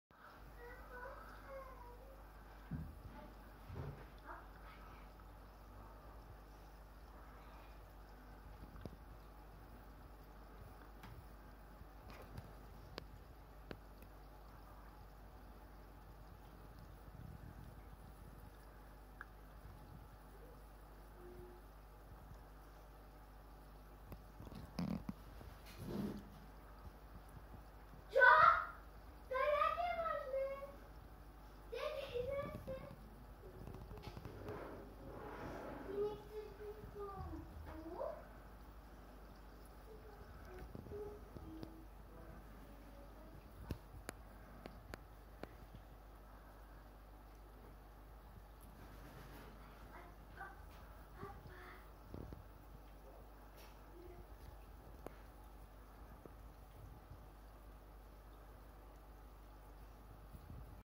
wf. Pt. Altum Orinoco Mataven caño castillo deň 3.
Ryby s chuťou prijímajú naliahnutú žiabronôžku soľnú.